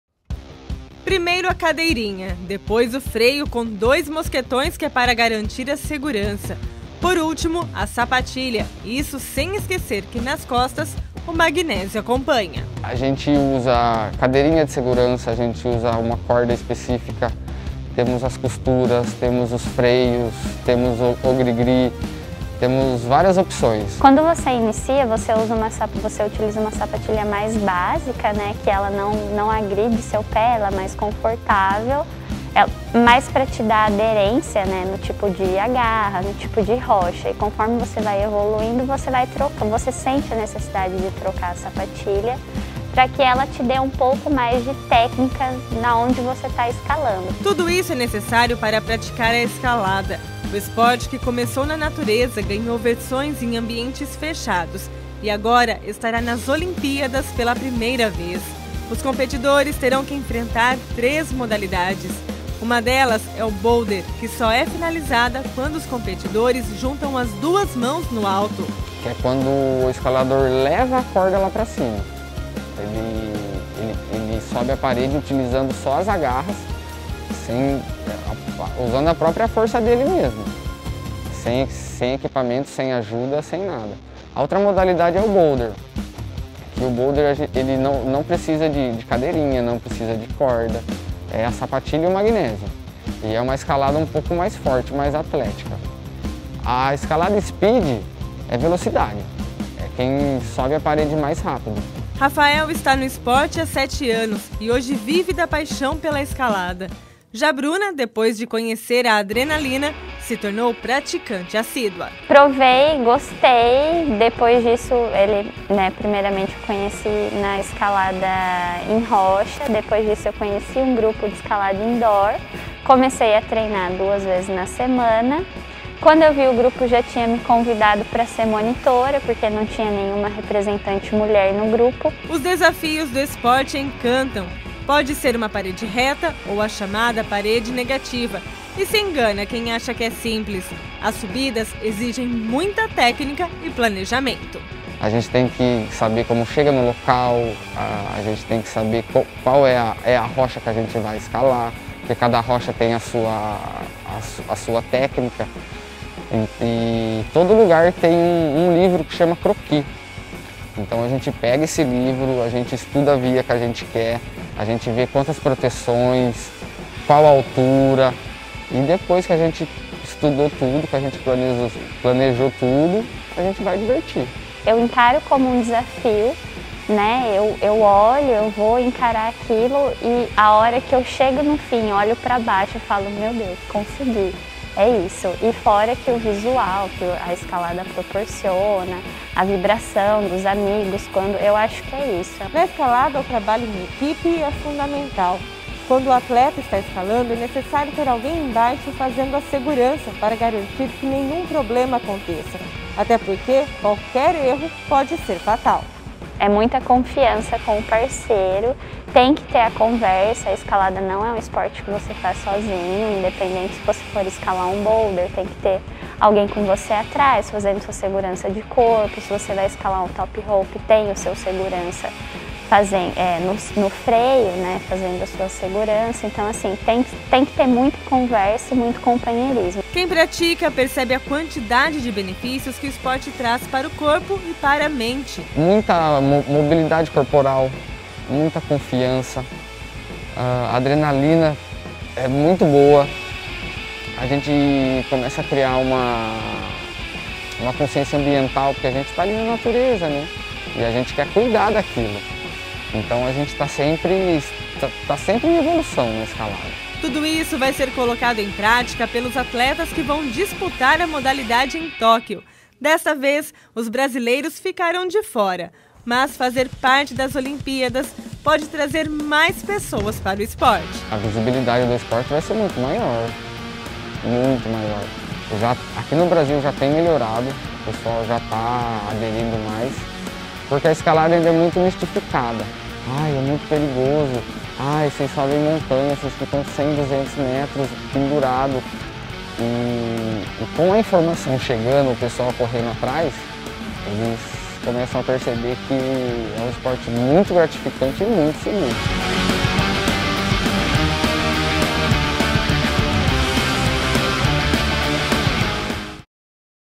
Escalada estreia como modalidade olímpica em Tóquio
Apesar de nenhum brasileiro estar na disputa, grupo já pratica o esporte em Limeira.